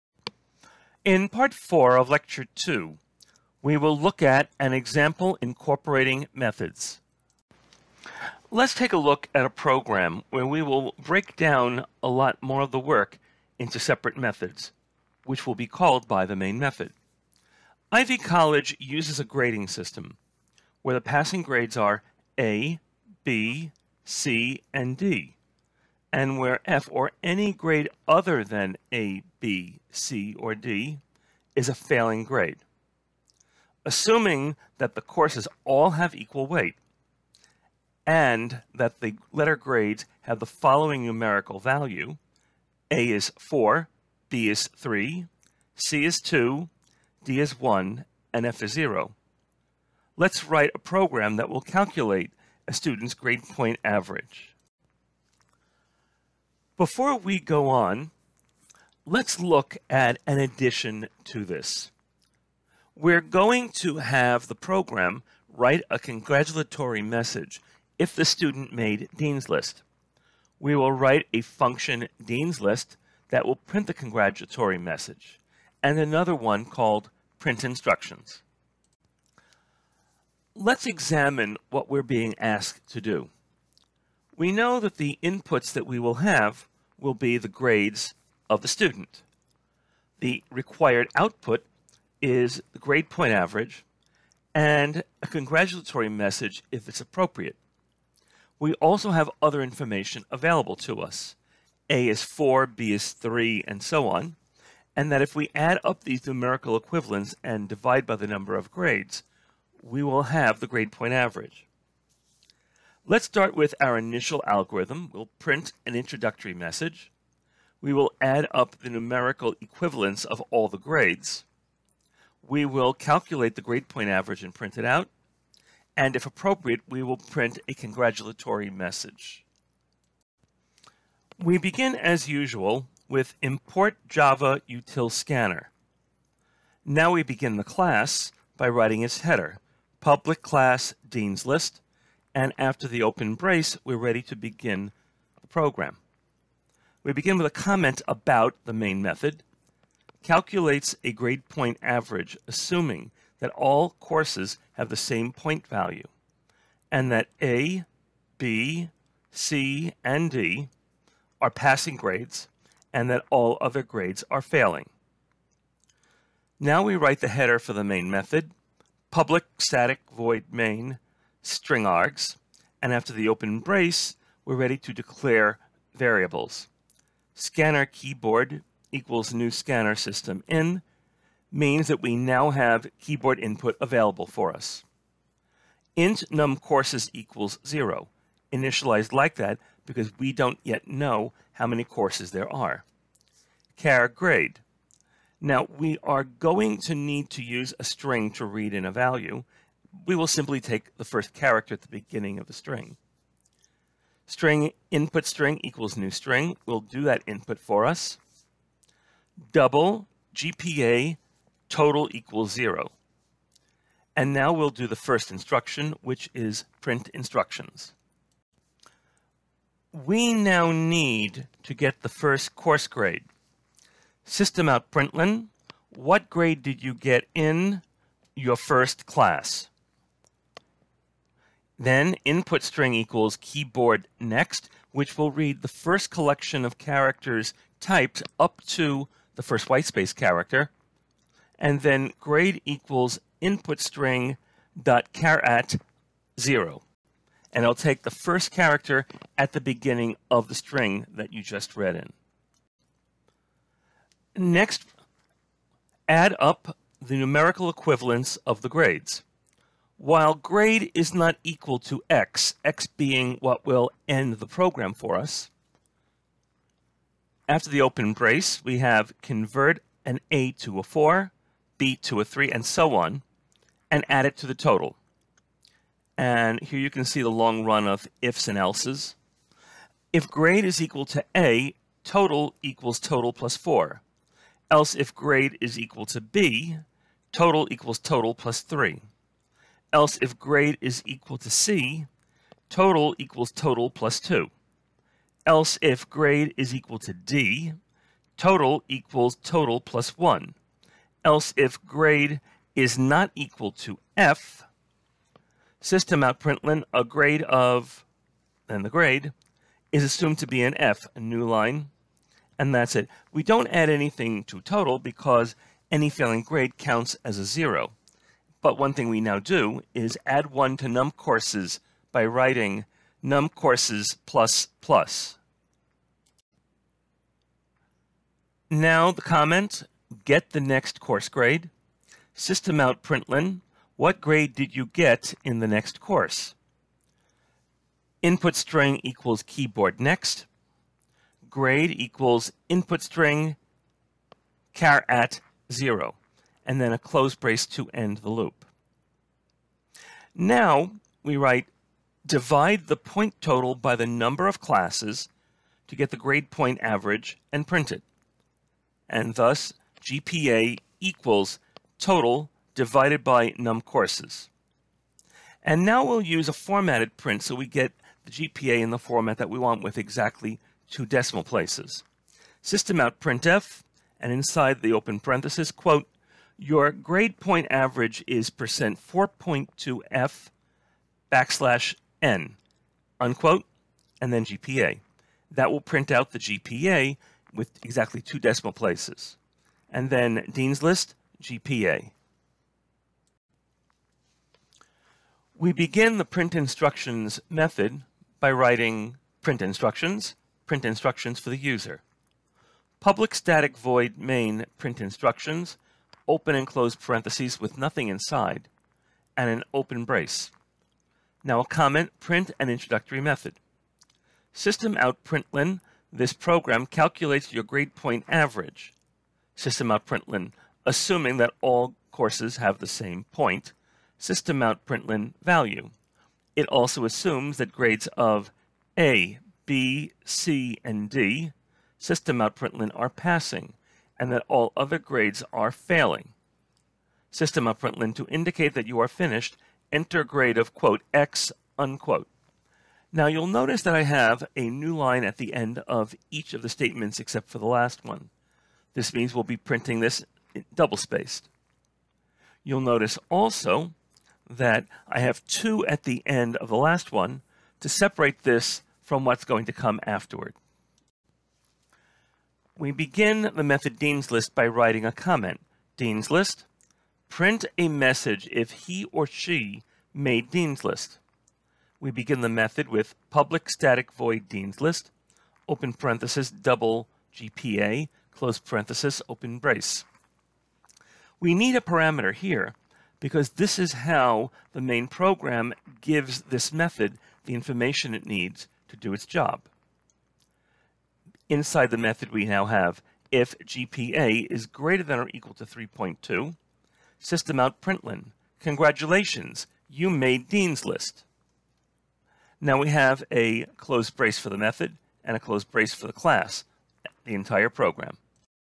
Intermediate Programming Lecture 2 Part 4
Conditional Loops and Modular Programming: An Example Incorporating Methods